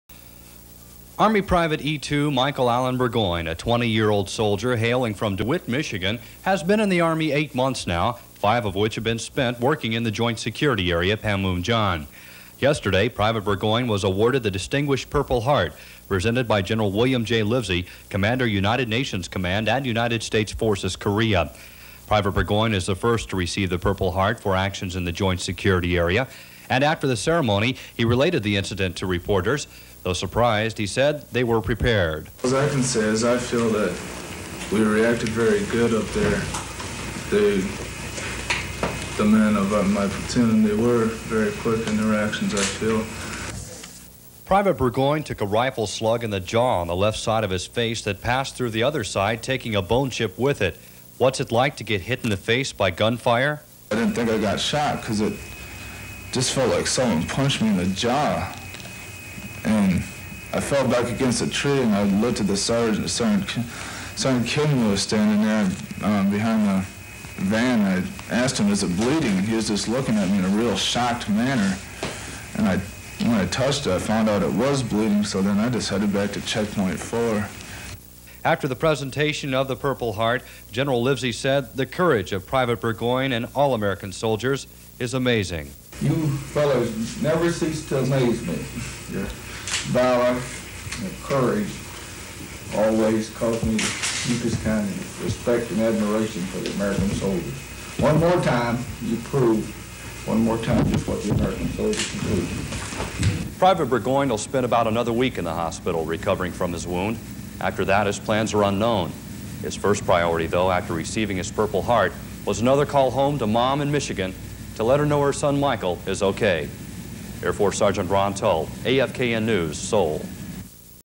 1984 Purple Heart US Army PFC Michael Burgoyne North Korea Attack Shooting JSA DMZ Panmunjom Soviet
American Forces Korea Network (AFKN) coverage of the awarding of the purple heart by to U.S. Army PFC Michael Burgoyne, shot and injured in a firefight by North Korean soldiers in the Joint Security Area, or JSA, during the defection of a Soviet citizen.  The soldier received the medal from U.S. Army Gen. William J. Livsey, commander of the American-led United Nations Command, at the 121st U.S. Evacuation Hospital in Seoul where he was recuperating from a gunshot wound in his jaw.

From Wikipedia: "On November 23, 1984, during a communist-led tour, Soviet citizen Vasily Matuzok (sometimes mistakenly spelt as Matusak) suddenly dashed across the Military Demarcation Line into South Korea. 30 Korean People's Army or KPA soldiers pursued him, firing their weapons as they did so. The Joint Security Forces or JSF commanded by Captain Bert Mizusawa deployed from Camp Kitty Hawk (renamed Camp Bonifas in August 1986) to safeguard Matuzok and repel the North Koreans. The KPA soldiers, who were pinned down by fire from the JSF's 4th Platoon on guard duty in Panmunjom, were quickly outmaneuvered and isolated in the area of the Sunken Garden, now the site of the Unification Monument. In the 40-minute firefight that ensued, Corporal Jang Myong-ki was killed, and Private First Class Michael A. Burgoyne was wounded. The JDO NCO negotiated a ceasefire that enabled the North Koreans to withdraw, but not before five of them were wounded and three killed, plus an additional eight captured. It has been rumored that Lt. Pak Chul ("Lt. Bulldog," who commanded North Korean soldiers in the confrontation that led to the Axe murder incident a decade earlier) was one of those killed in this firefight, though documentation has not been found yet. However, he has not been seen in the JSA since this incident. Additionally, there were gunshots in the north approximately 20 minutes after the cease fire was effective, and it was reported by senior members of the Neutral Nations Supervisory Commission to the JSF commander that the KPA commander and one of his key subordinates were summarily executed. The KPA commander was likewise never seen again, however no evidence is provided for these claims. Matuzok was given a short debriefing in Seoul, then turned over to the UN High Commissioner for Refugees, which made arrangements for him to be resettled in the U.S. under the sponsorship of the International Rescue Committee, an organization that helps to resettle refugees in Western countries.